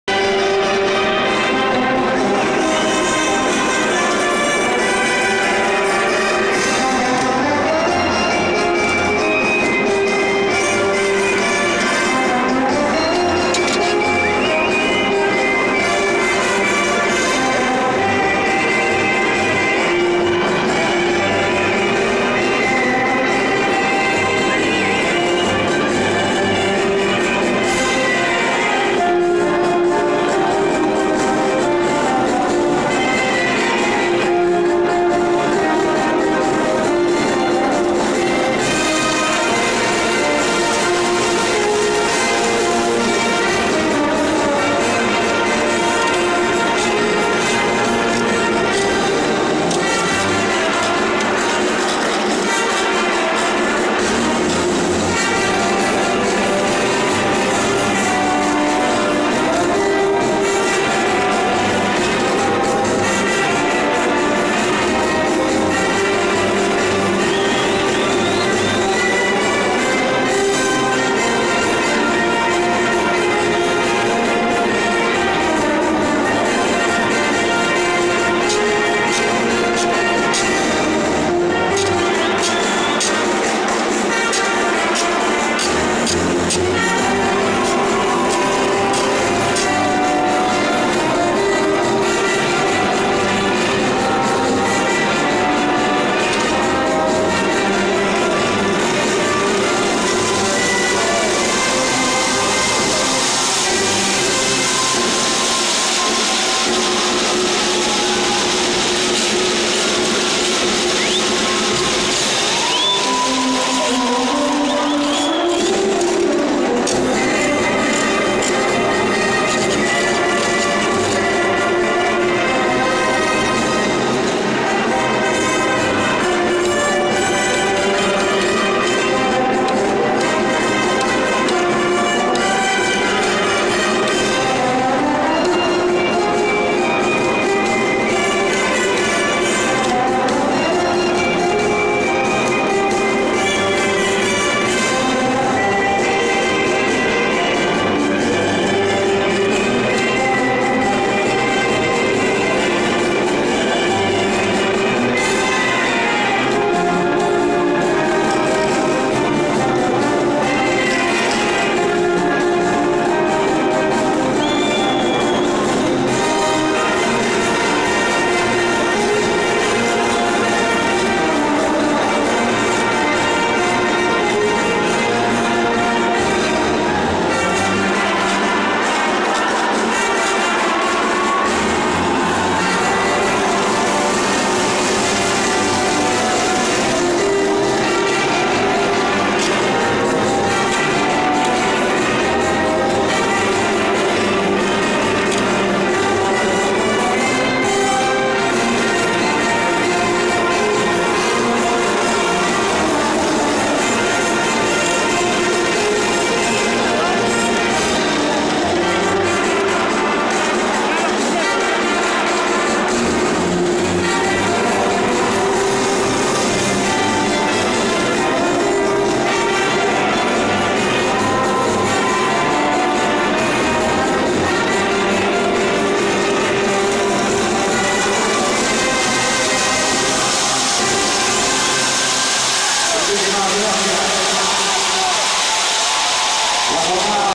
Marinera Norteña - Trujillo 2017
Marinera Norteña (representative dance from the north of Peru) performed by Evelyn Zuñiga Medina and Fredy Villanueva Asmat
Concurso Nacional de Marinera Norteña Trujillo 2017
Categoría Seniors - Etapa Semifinal
Coliseo Gran Chimu - Jueves 26 de Enero de 2017